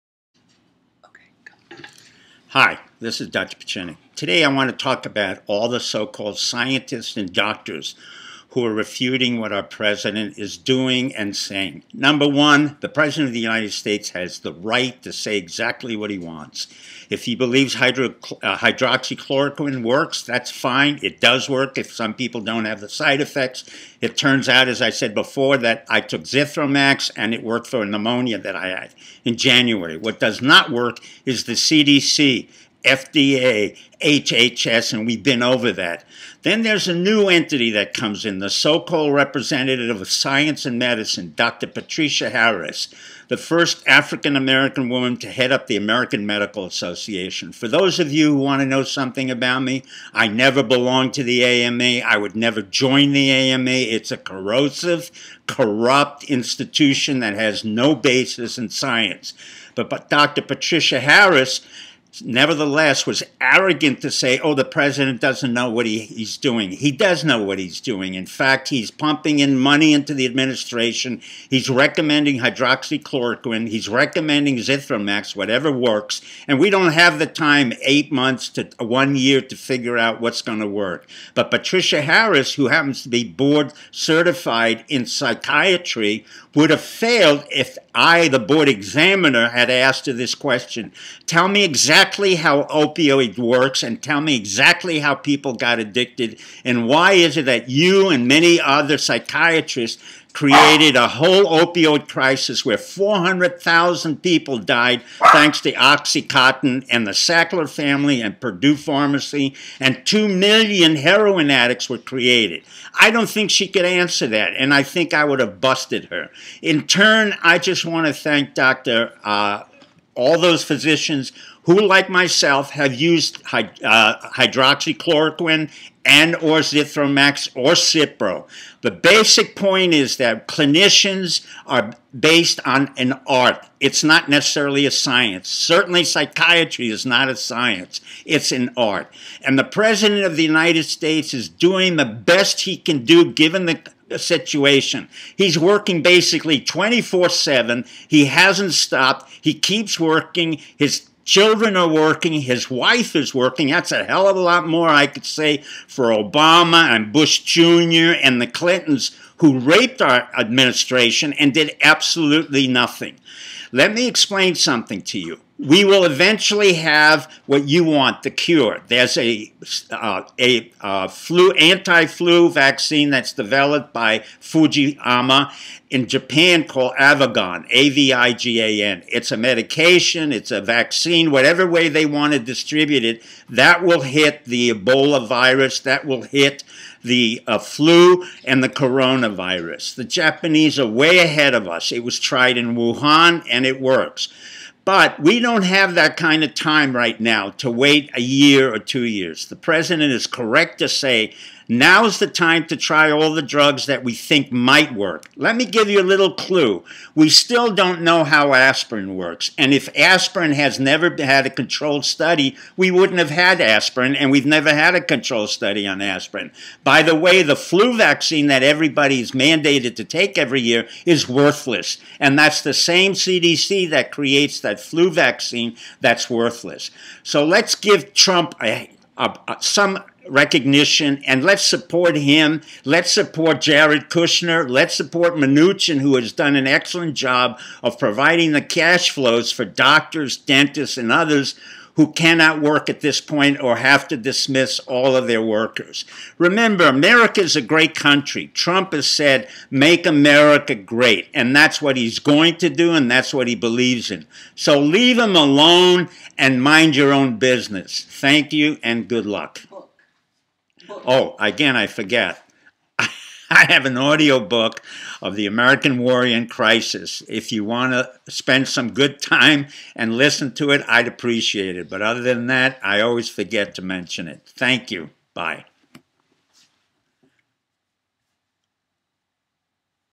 OPUS 212 Trump Leads thru Crisis! KAG
Thank God for Trump's steadfast leadership during Crisis especially considering the unjust treatment by Mockingbirdmedia and the quack doctors who r paid off by big pharma and worse...
I am a Board Certified Physician and I know a crisis when I see one